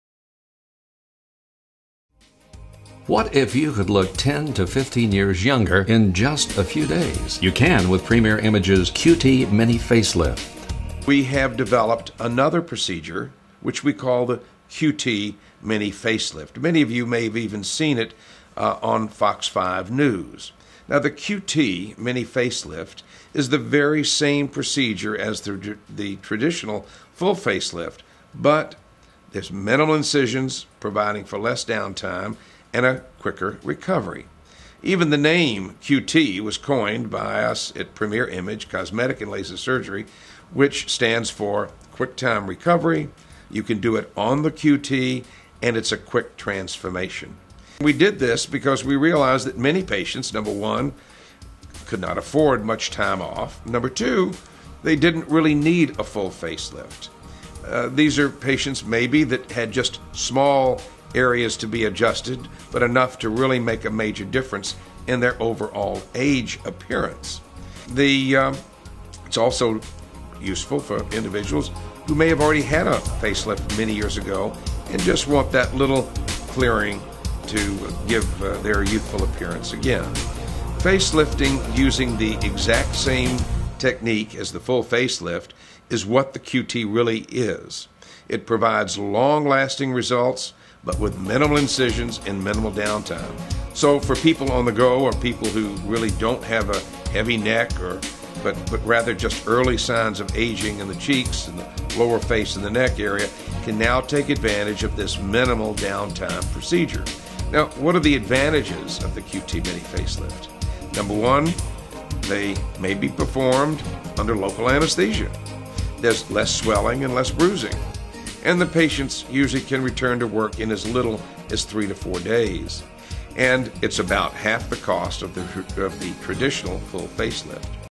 QT Mini Face Lift
Dr. William Silver discusses the QT Mini Face Lift developed exclusively by Premier Image Cosmetic & Laser Surgery.